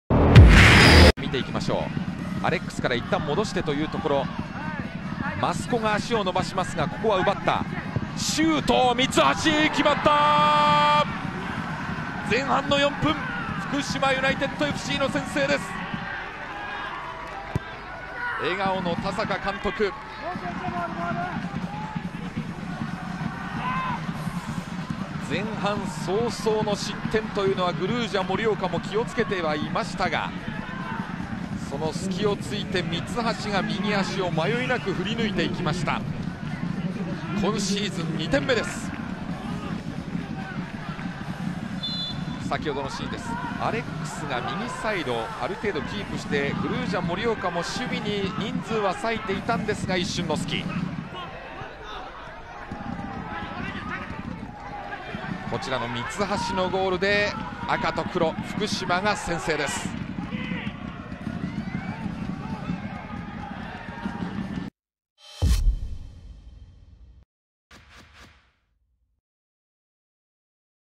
【公式】ゴール動画：三橋 秀平（福島）4分 グルージャ盛岡vs福島ユナイテッドＦＣ 明治安田生命Ｊ３リーグ 第14節 2017/6/25
三橋 秀平（福島）のダイレクトシュートがファーサイドに突き刺さり、試合開始早々に先制点を決める！明治安田Ｊ３のゴール動画を公開中！
2017年6月25日（日）に行われた明治安田生命Ｊ３リーグ 第14節 盛岡vs福島のゴール動画です。